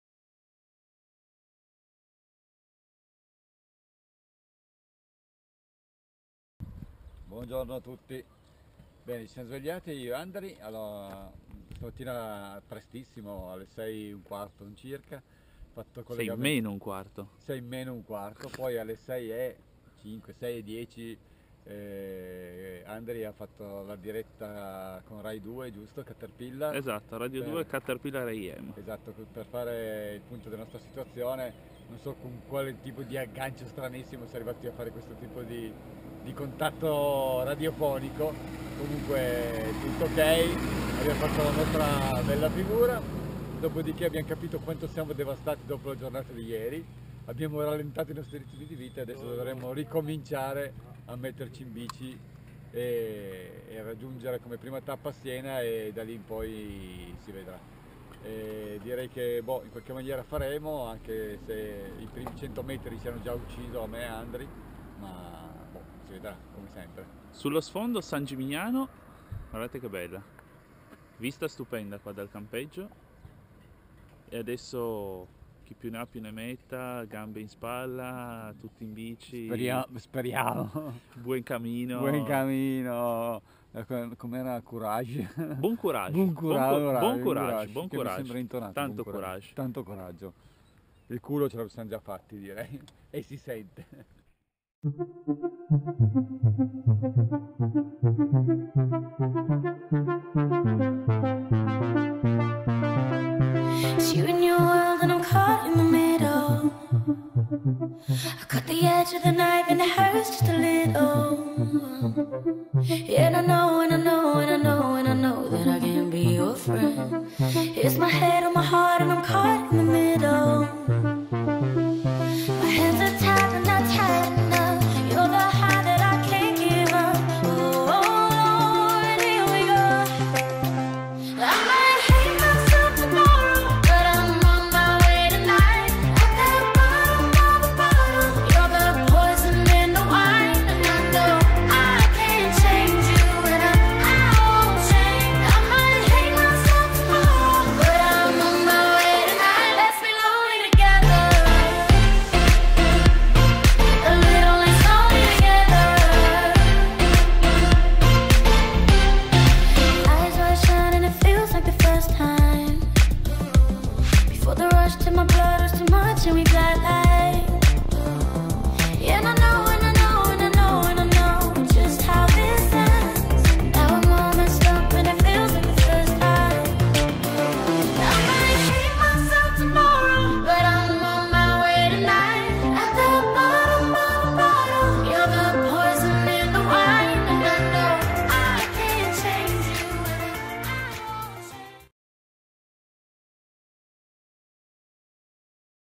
Ciclomaretona - Day 2 - Da San Gimignano a Siena
Seconda tappa del viaggio Coast to Coast dal Mar Tirreno al Mar Adriatico!